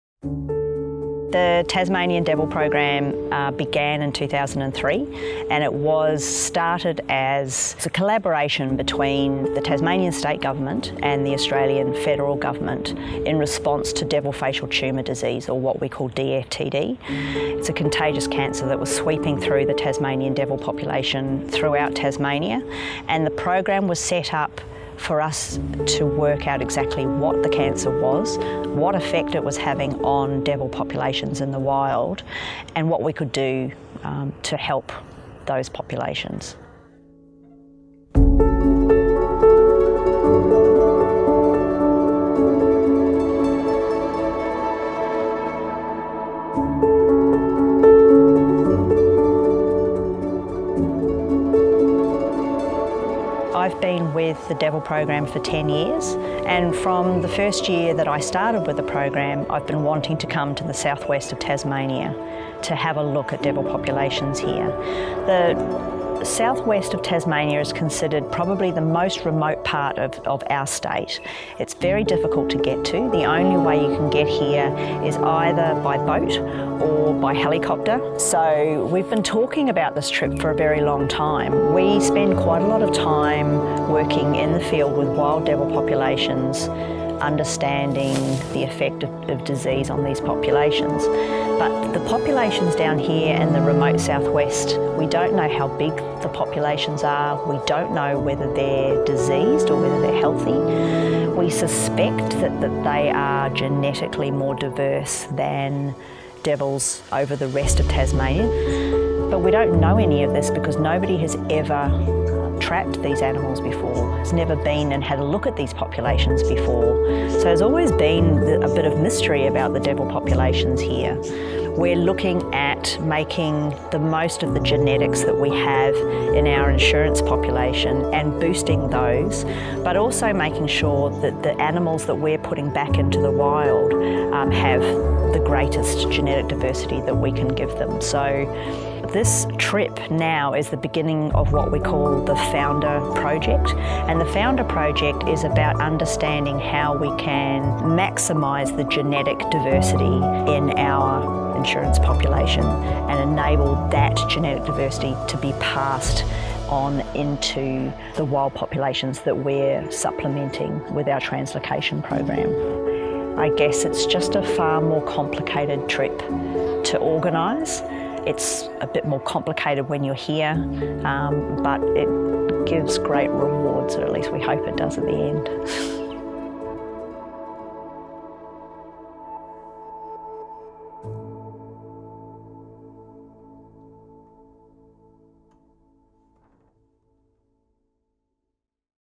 SW Tasmanian Devil Expedition | Toledo Zoo
Here's a preview of some recent work we have done with our partners from Save the Tasmanian Devil Program and the University of Sydney to study Tasmanian Devil populations in remote southwest Tasmania!